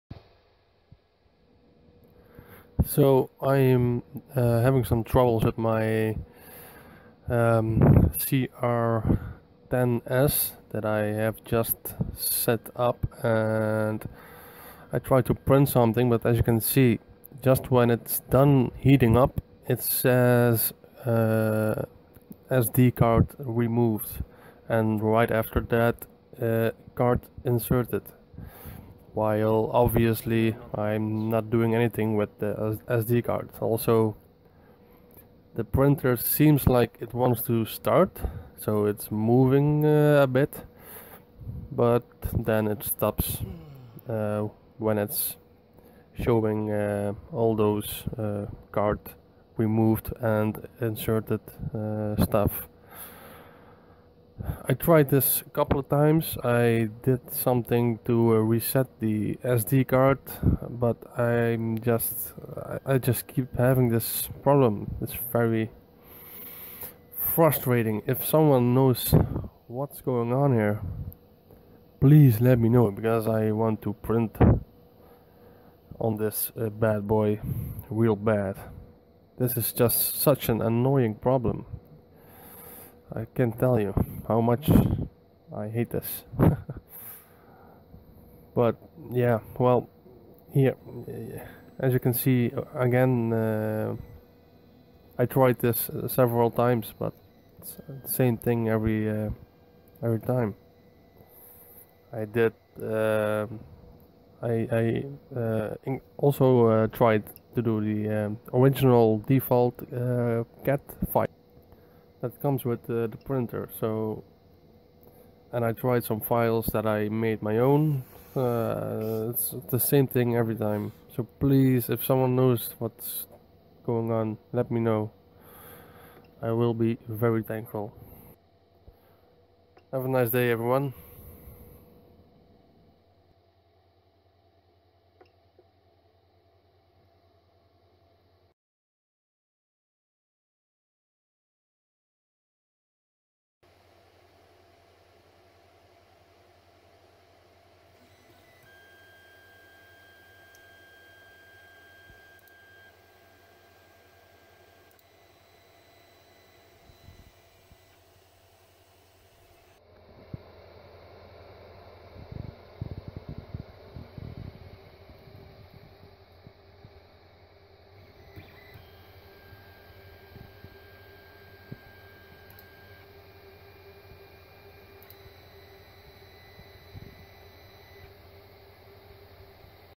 Just got my CR-10s 3d printer - Troubles right out of the box...
EDIT: Just noticed in the video.. that is says "card removed" when it hit's the first safety switch, and then "inserted" again when it hits A second safety.. so it looks gelated to those switches...

Just got my Creality CR-10s.. And it has troubles involving reading the SD-card.. Or so it looks like.. VERY frustrating.. So if anyone knows what's up with this.. PLEASE let me know. Would be very much appreciated! Cheers